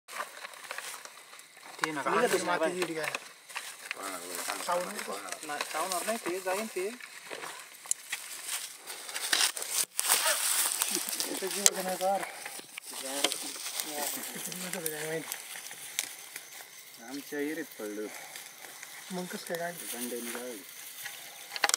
Rescued Civet Cat released by Wildlife SOS
An Indian Civet Cat was rescued from drowning in a 20-foot deep wellI in Khamgaon village, Junnar, Maharashtra. The civet was safely extricated from the well with the help of a transport cage, and was released back into the wild once deemed fit.